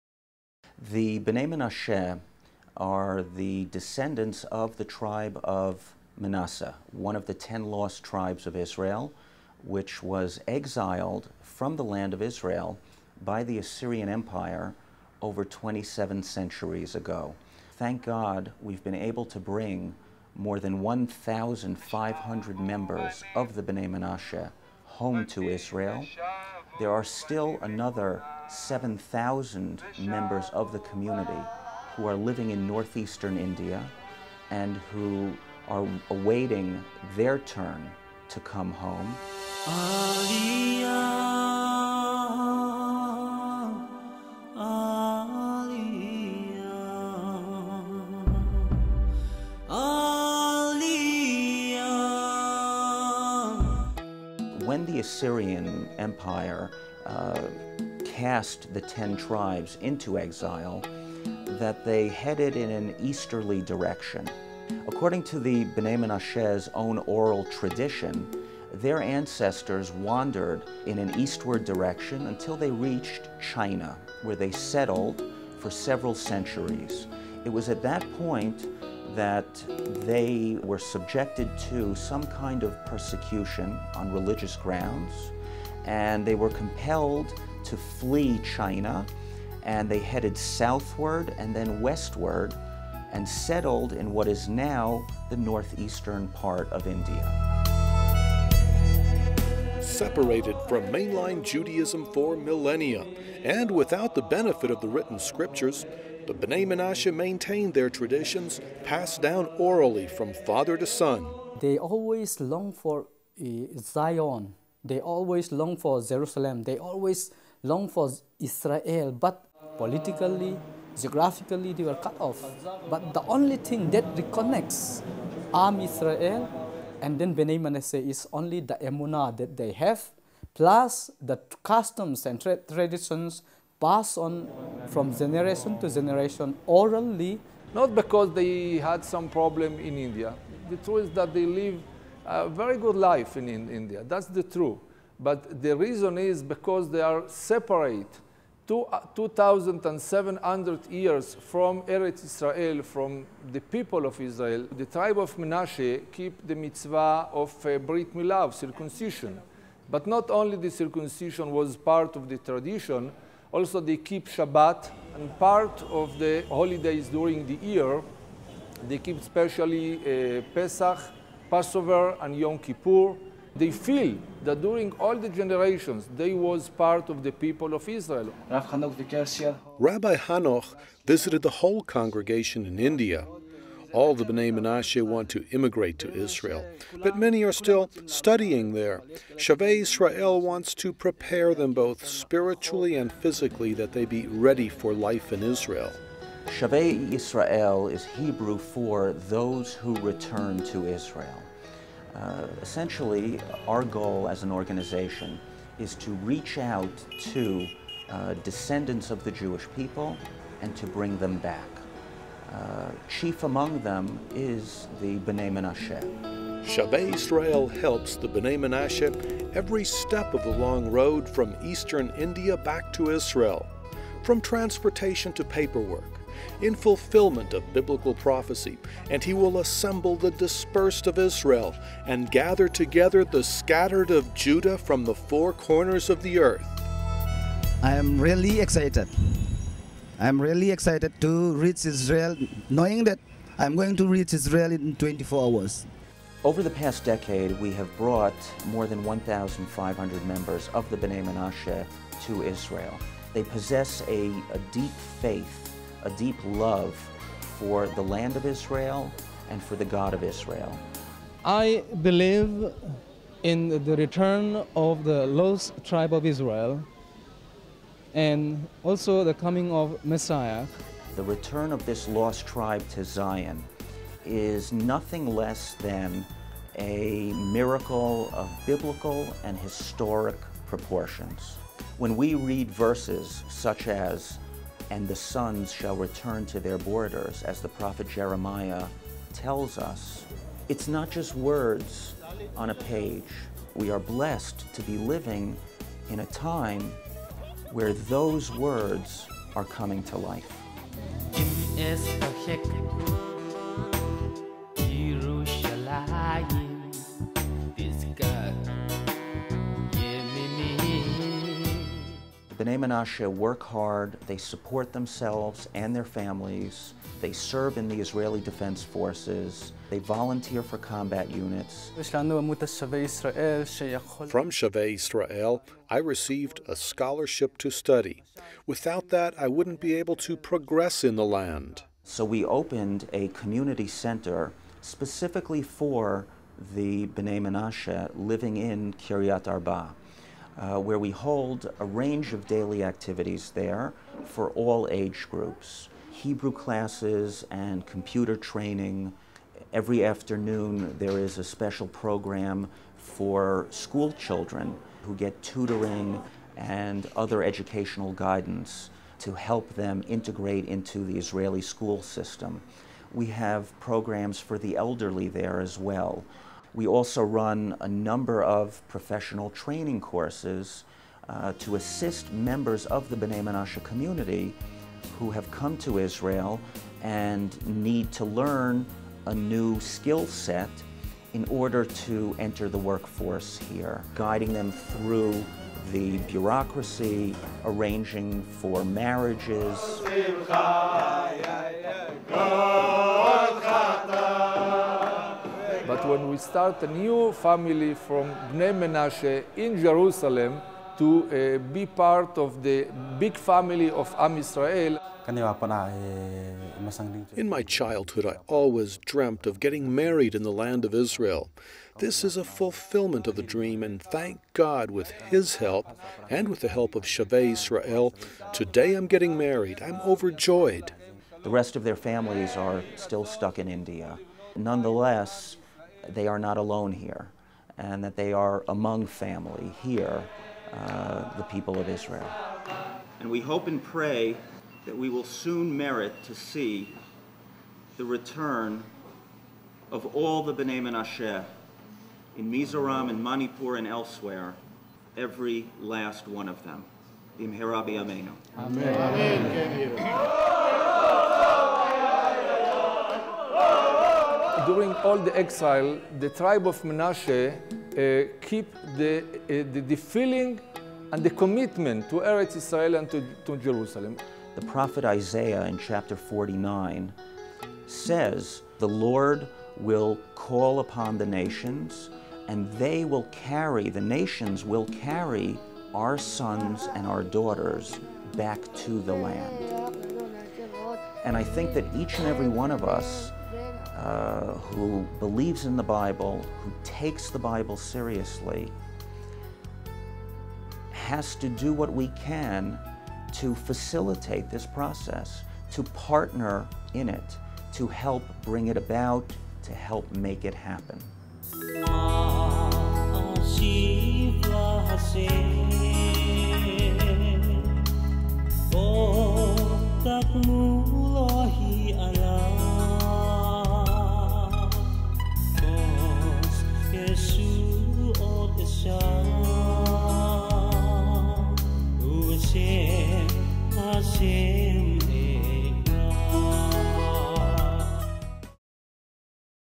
Bnei Menashe Documentary - from India to Israel
A fast-paced, moving documentary on the Bnei Menashe community in India and those who have made aliyah through Shavei Israel